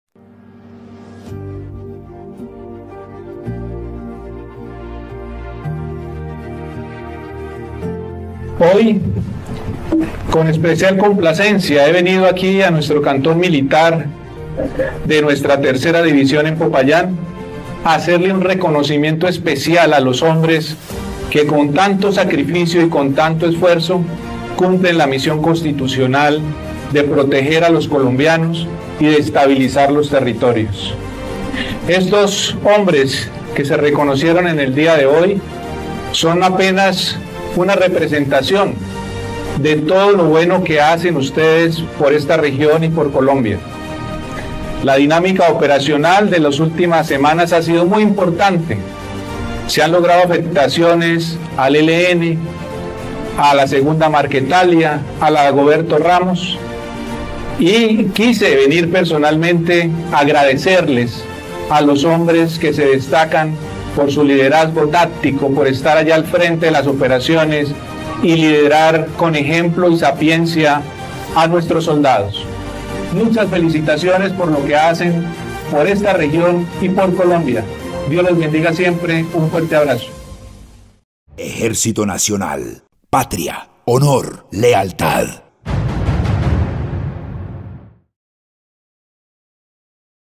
El Comandante del Ejército Nacional reconoce la labor de los soldados de la Tercera División
nuestro comandante del Ejército , el General Luis Emilio Cardozo Santamaría hizo un reconocimiento a los soldados que día a día trabajan por la seguridad de Las habitantes de la región del Cauca.